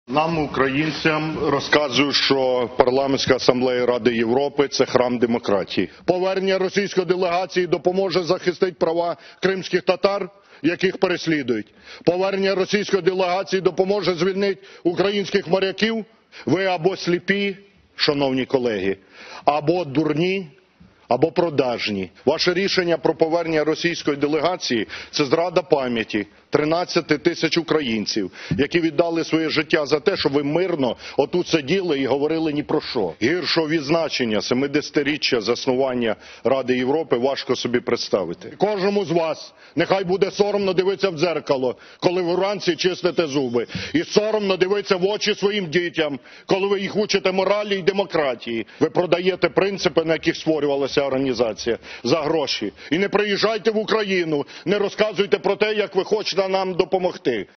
Ляшко-євродепутатам: Ви або сліпі, або дурні, або продажні!
Під час розгляду питання про повернення російської делегації до ПАРЄ Олег Ляшко нагадав європейцям, що 13 тисяч українців віддали свої життя у протистоянні з Росією. Лідер Радикальної партії назвав аргументи для повернення росіян надуманими і наголосив, що європейці продають принципи ПАРЄ за гроші росіян.